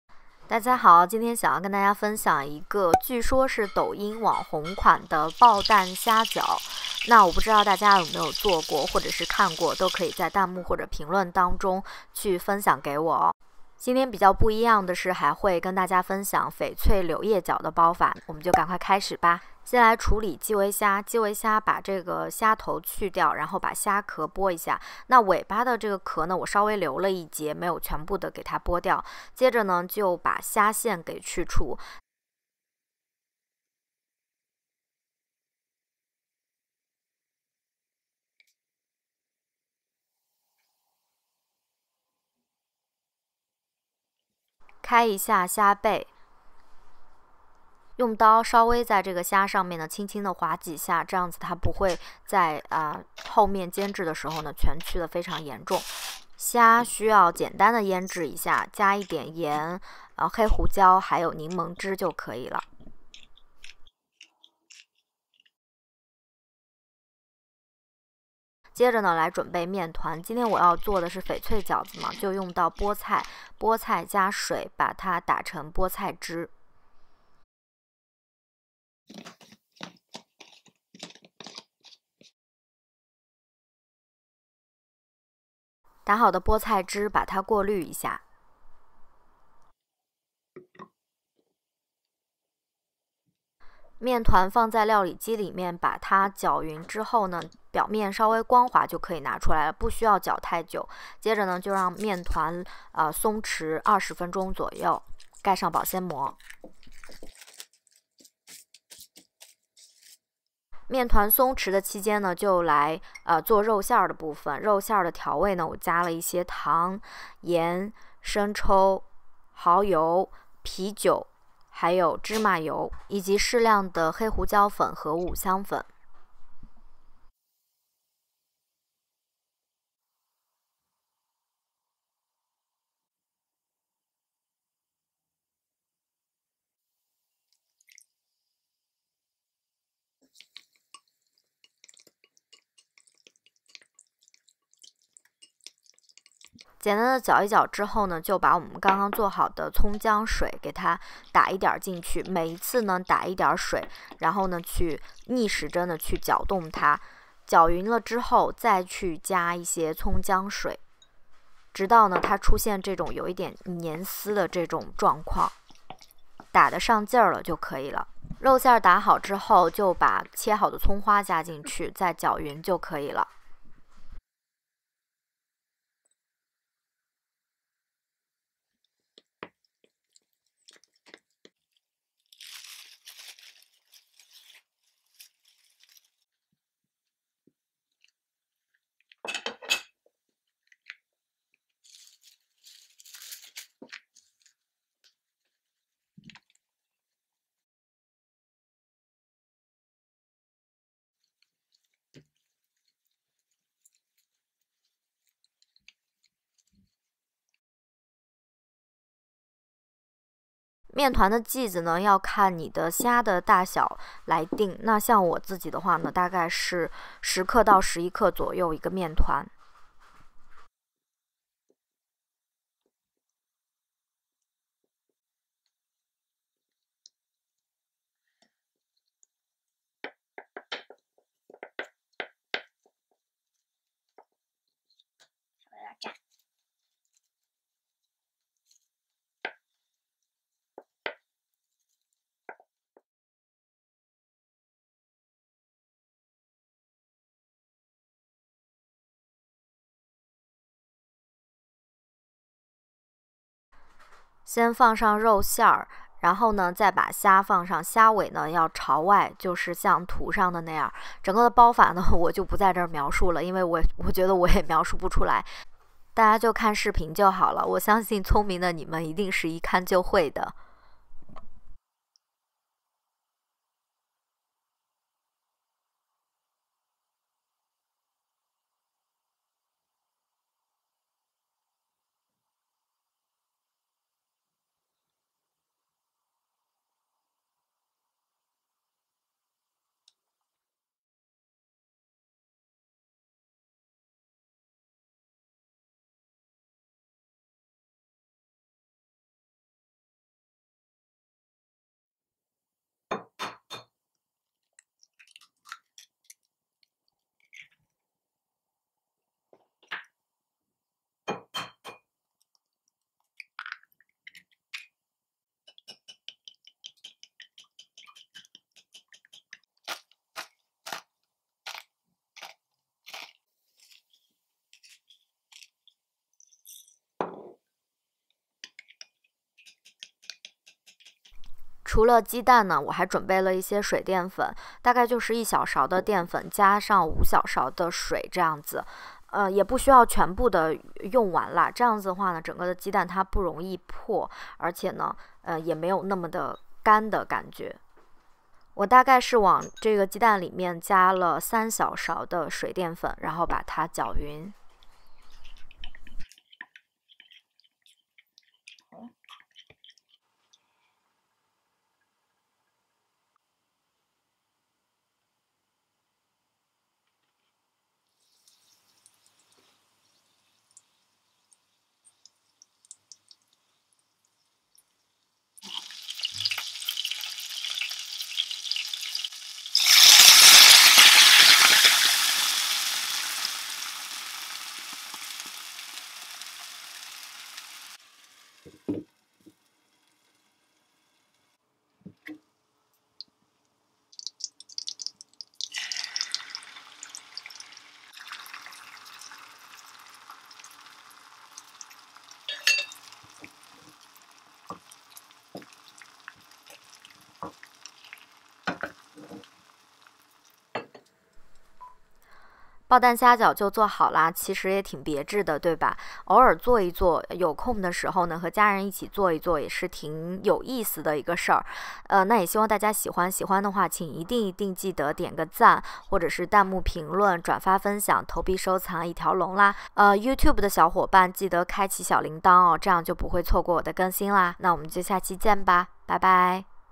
纯手作翡翠抱蛋煎饺 | 内含柳叶饺包法 | 虾饺 | Pan fried Shrimp dumplings with eggs
这样的抱蛋煎饺你萌吃过嘛?视频绝对干货！从条馅儿、做饺子皮、清新秀气的柳叶饺包法、到水煎包过程和如何做出滑嫩好吃但不破皮的鸡蛋等一站式分享。

还有更多好内容，大家记得关注，不要错过哟~分享给最棒的你们